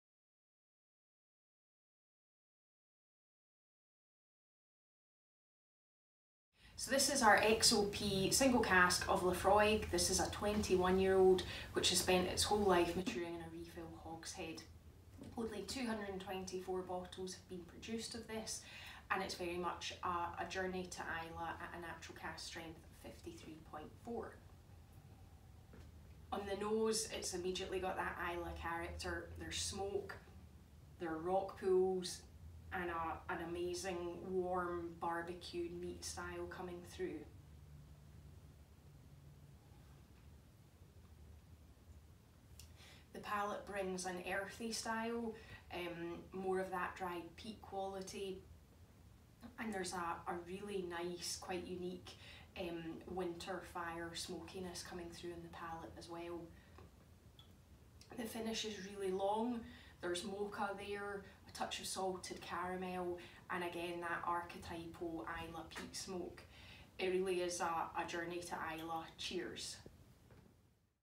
XOP Laphroaig 21 Years Old Whisky Tasting Douglas Laing DL13523
Cara Laing, Director of Whisky, shares her tasting of XOP Laphroaig 21 Years Old. This release is bottled at 53.4% Natural Cask Strength and there are only 224 bottles in existence.